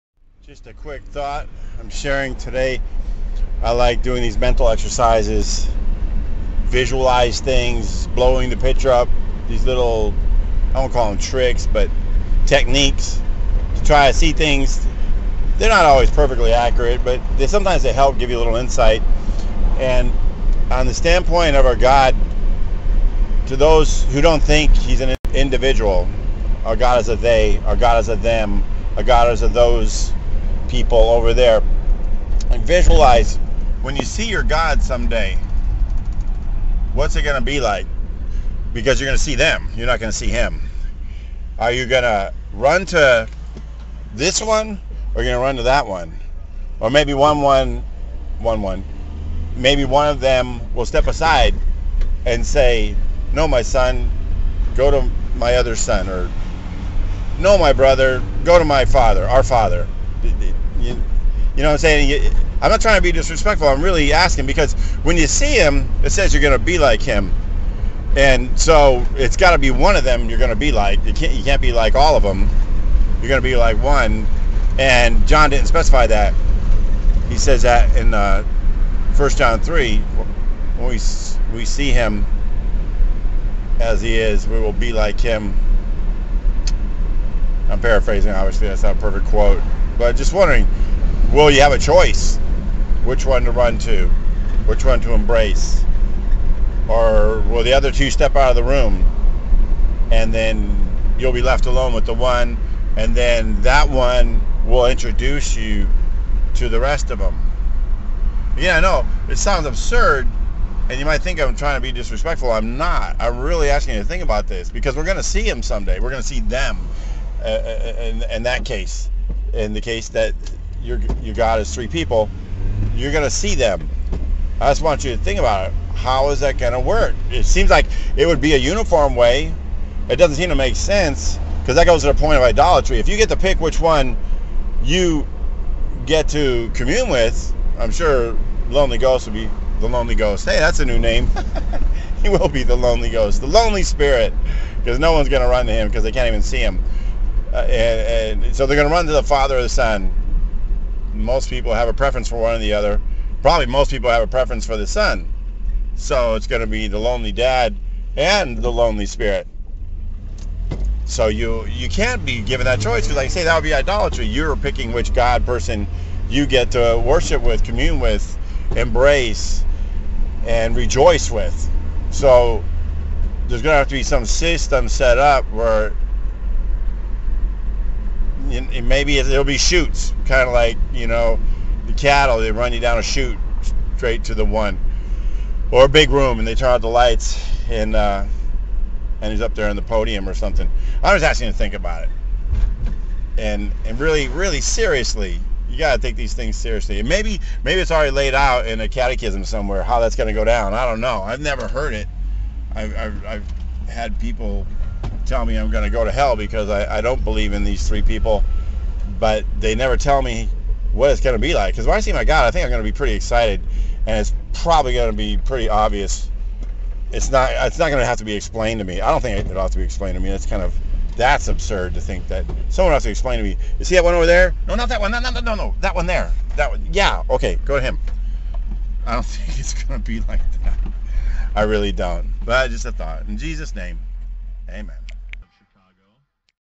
When You See Him As He Is - Which 1 of "Them" Will We Be Like?
Behold, what manner of love the Father hath bestowed upon us, that we should be called the sons of God: therefore the world knoweth us not, because it knew HIM not. Beloved, now are we the sons of God, & it doth not yet appear what we shall be: but we know that, when HE shall appear, we shall be like HIM; for we shall see HIM as HE is. And every man that hath this hope in HIM purifieth himself, even as HE is pure. (1 John 3:1-3 KJV)